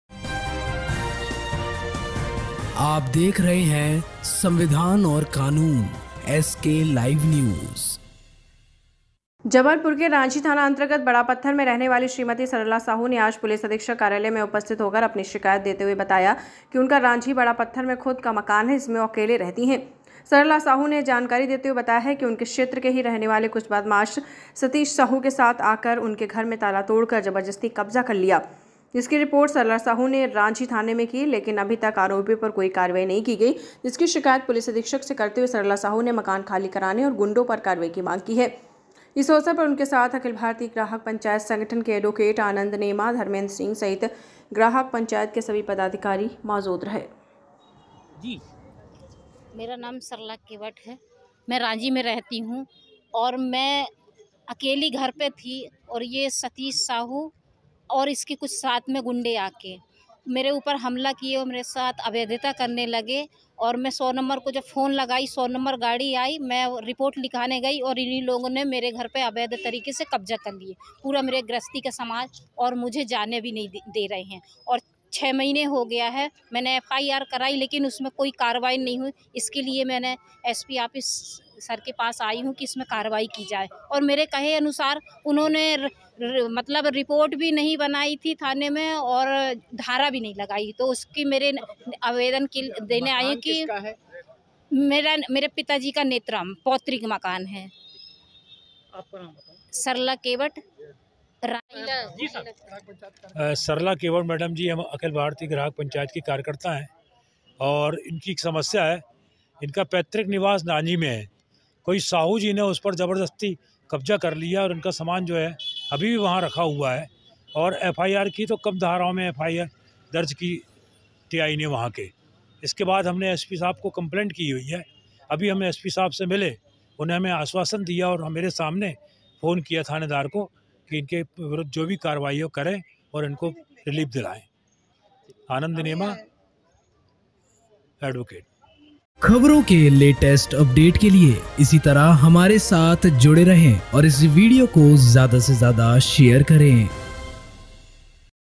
जबलपुर के रांझी थाना अंतर्गत बड़ा पत्थर में रहने वाली श्रीमती सरला साहू ने आज पुलिस अधीक्षक
जबलपुर के रांझी थाना अंतर्गत बड़ा  पत्थर में रहने वाली श्रीमती सरला साहू ने आज पुलिस अधीक्षक कार्यालय  में उपस्थित होकर अपनी शिकायत देते हुए, बताया कि उनका रांझी बड़ा पत्थर में खुद का मकान है ,जिसमें वह अकेले रहती हैं, सरला साहू ने जानकारी देते हुए, बताया कि उनके क्षेत्र के ही रहने वाले कुछ बदमाश सतीश साहू के साथ आकर उनके घर में ताला तोड़कर जबरदस्ती कब्जा कर लिया है, जिसकी रिपोर्ट सरला साहू ने रांझी थाने में की ,लेकिन अभी तक आरोपियों पर कोई भी कार्यवाही नहीं की गई है, जिसकी शिकायत पुलिस अधीक्षक से करते हुए सरला साहू ने मकान खाली कराने और गुंडो पर कार्रवाई की मांग की है ,इस अवसर पर उनके साथ अखिल भारतीय ग्राहक पंचायत संगठन के एडवोकेट आनंद नेमा,एवं धर्मेंद्र सिंह ,सहित ग्राहक पंचायत के सभी पदाधिकारी कार्यकर्ता उपस्थित थे